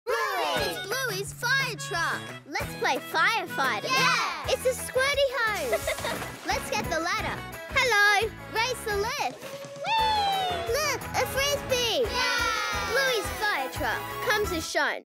Bluey I Bluey Firetruck TVC I 15
Join Bluey and Bingo on a brand new adventure with the Bluey Firetruck! This action-packed vehicle has so many fun features for kids to play with. The firetruck comes with 2 figures, Firefighter Bingo and Bluey. It even includes a Bob Bilby accessory that fits on Bluey's hand! The firetruck is just the right size so Bluey and Bingo can sit in the truck's cabin and ride along. Raise the built-in ladder and spin it around! Place Bingo on top of the ladder and move the water hose to help her aim! This awesome firetruck also has a retractable hose on the side that kids can reel out for more action. On the other side of the truck is a removable ladder that simply slides out. There is even a sticker sheet to add to the fun! Both Bluey and Bingo figures are articulated with moving arms and bodies and are just the right size for pre-school hands to play with. Toddlers will love to recreate some of the most memorable moments from the show with this great toy!